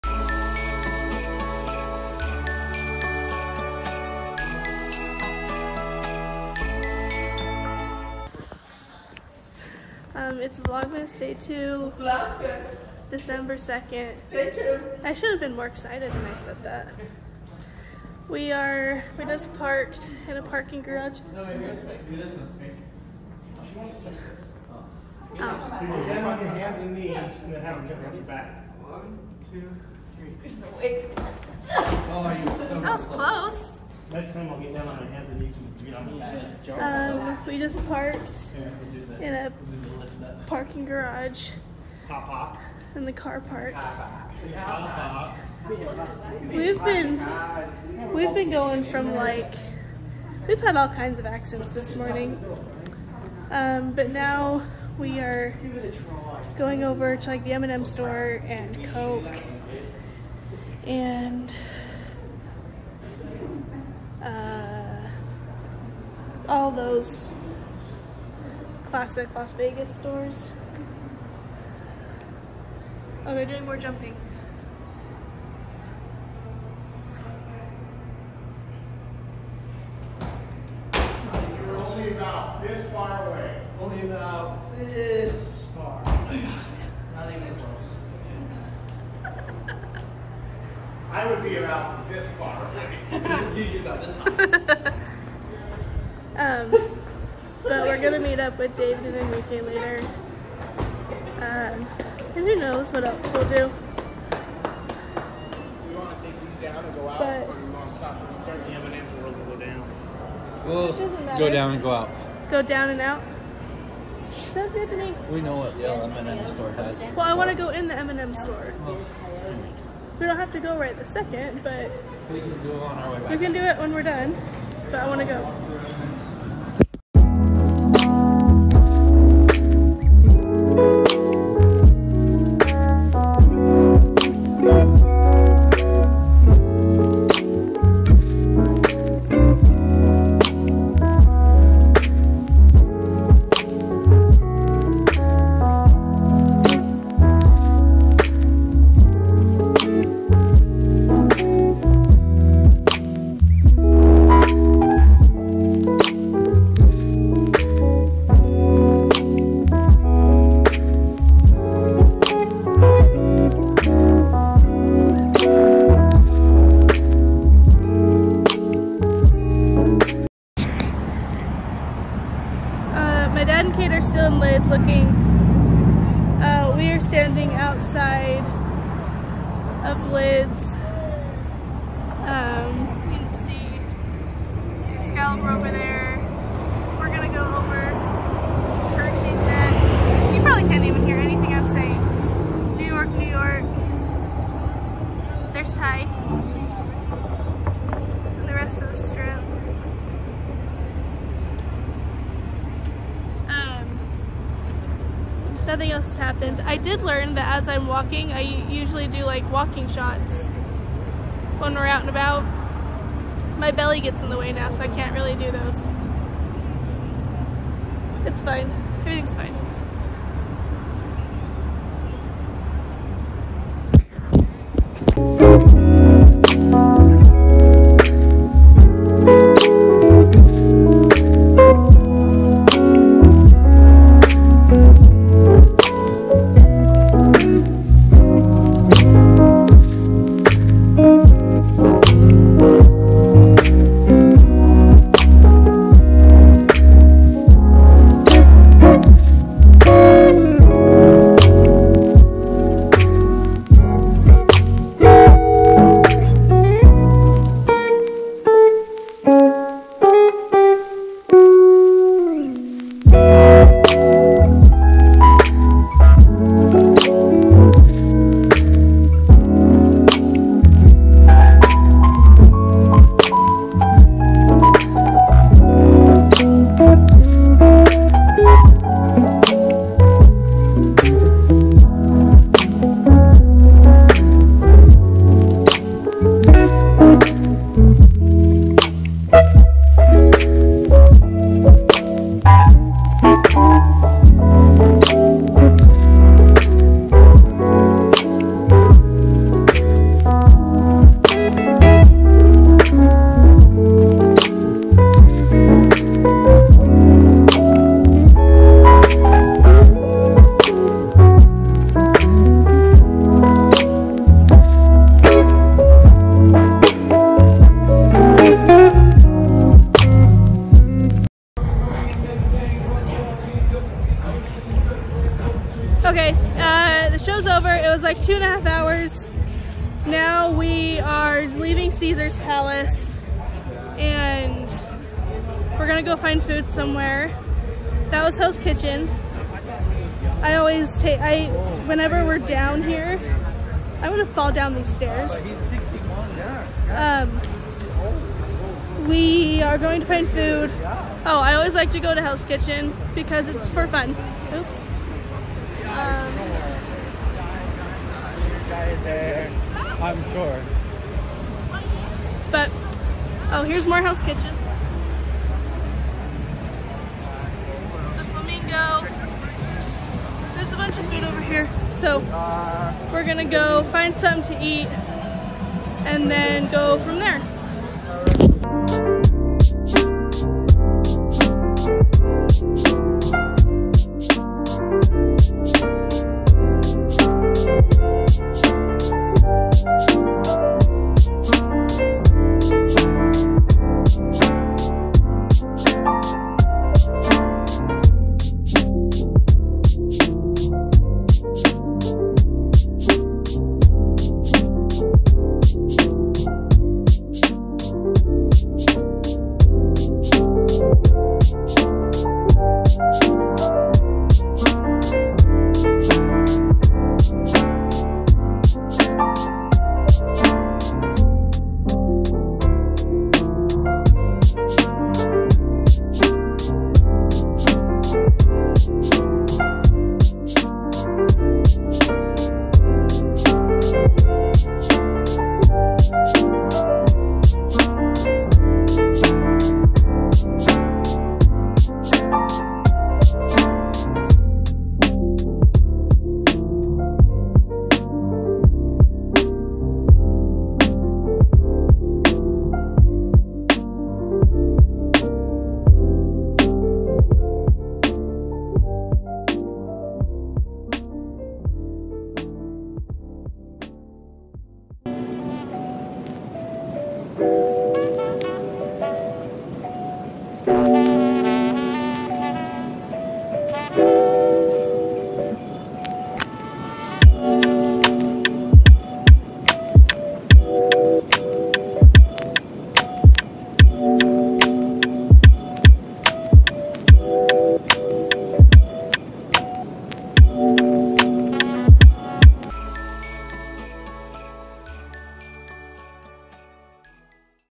VLOGMAS: December 2 & 3
We go down the strip and make our way to see Garth Brooks,